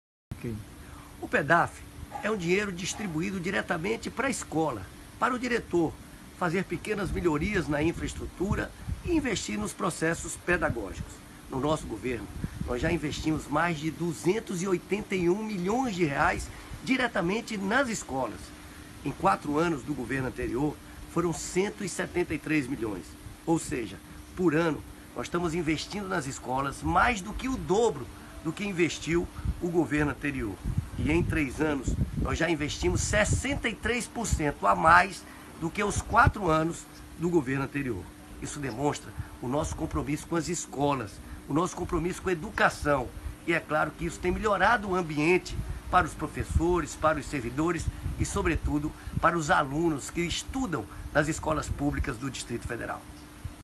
Recursos do PDAF para escolas públicas
No nosso governo, já investimos mais de R$ 281 milhões em escolas públicas com recursos do PDAF. Por ano, é mais do que o dobro do que investiu a gestão passada